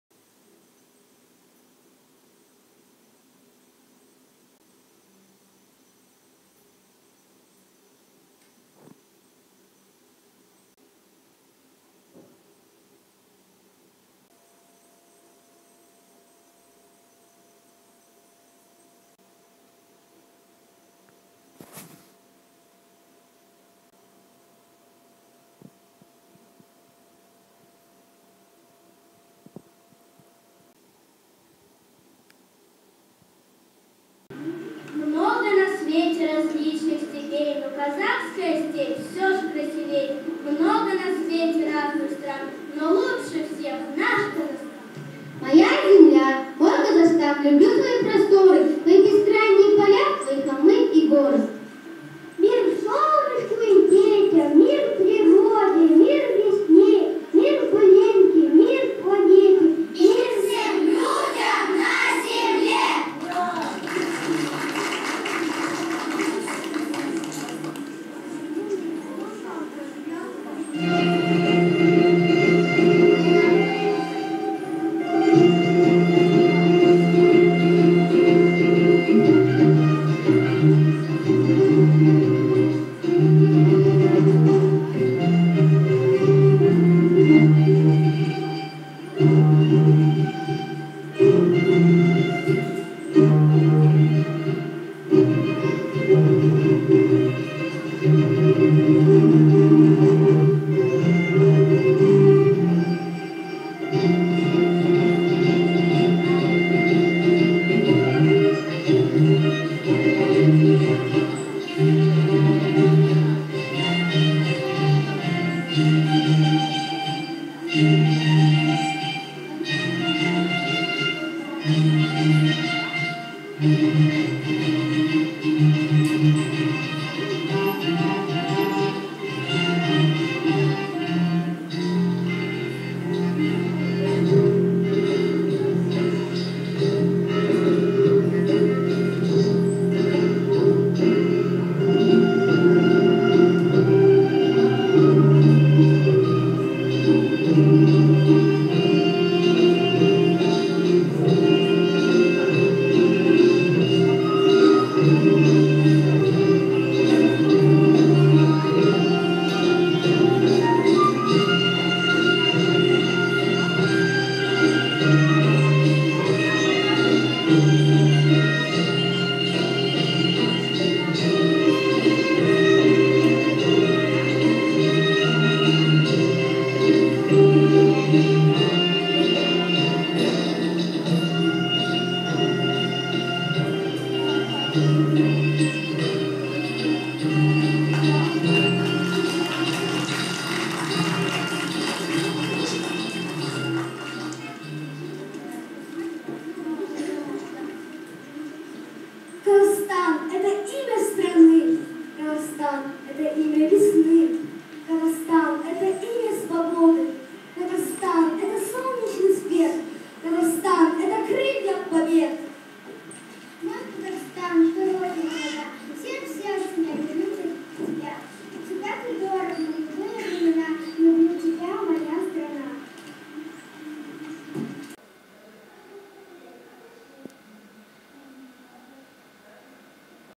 Танец с веерами :) Я, Аня и Юля:)
3 класс 11 декабря 2012 год. С днем рождения Казахстан!!!  День независимости республики Казахстан!!!